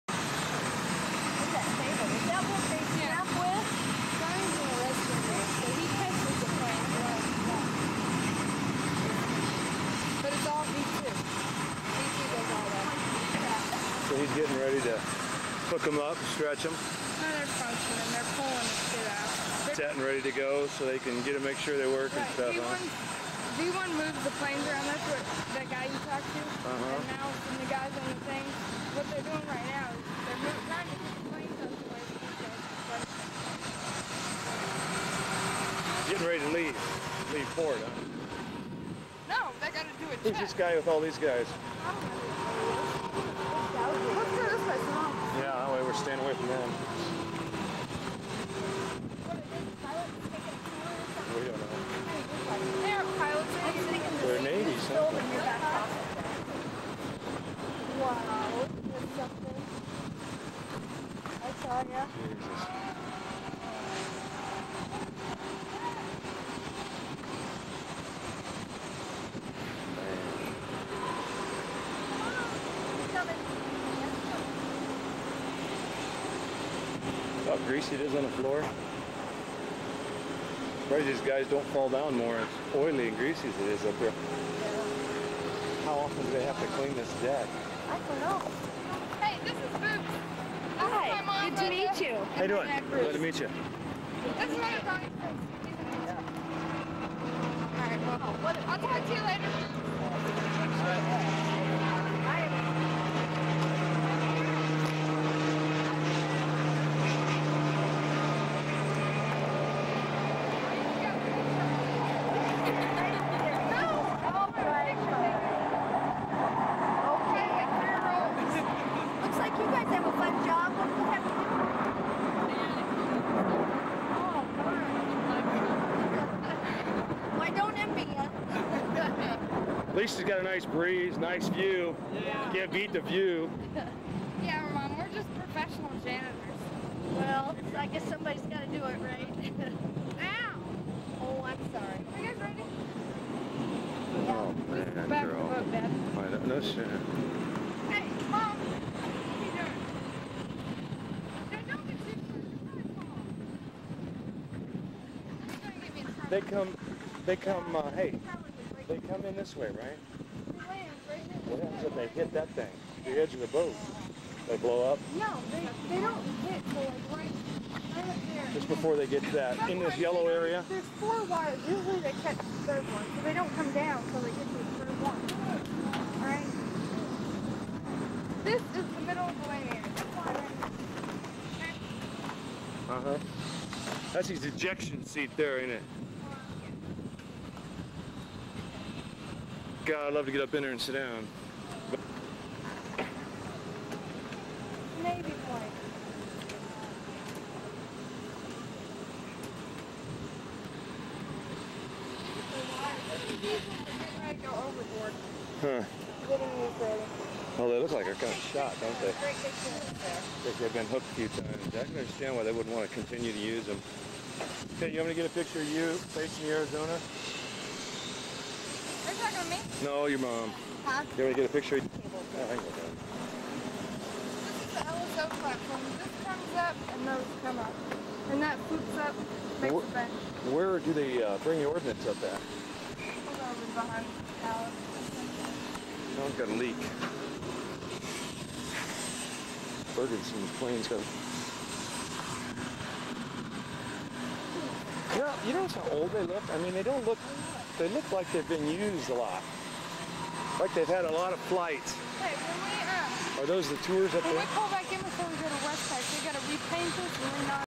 Visiting the Full Flight Deck of the USS Abraham LIncoln CVN-72 RIMPAC 2000
USS Abraham Lincoln CVN-72 flight deck full of planes and lots of work going on around them during RIMPAC 2000. arresting cable repairs being done by cat crew and lots of other actions going on getting ready for manuevours around Hawaiian islands